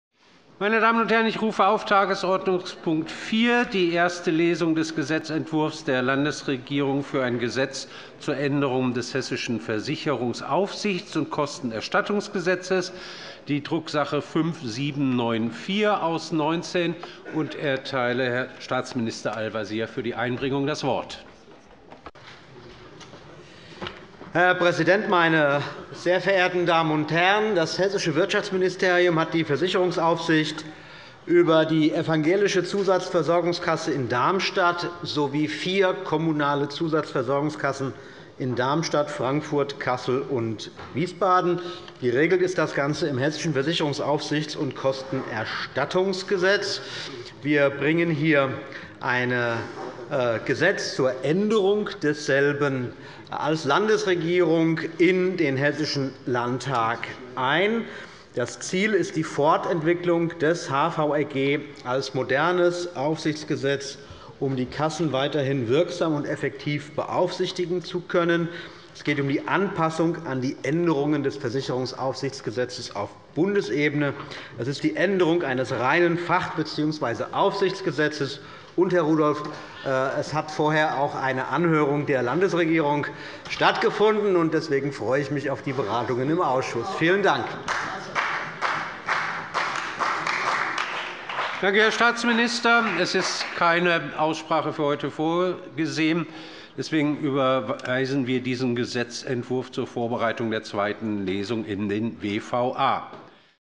Änderung des Versicherungsaufsichts und Kostenerstattungsgesetzes - 30.01.2018 - 126. Plenarsitzung
0:23 Staatsminister Tarek Al-Wazir, MdL, Hessischer Minister für Wirtschaft, Energie, Verkehr und Landesentwicklung

Erste Lesung des Gesetzentwurfs der Landesregierung für ein Gesetz zur Änderung des Hessischen Versicherungsaufsichts- und Kostenerstattungsgesetzes – Drucks. 19/5794 –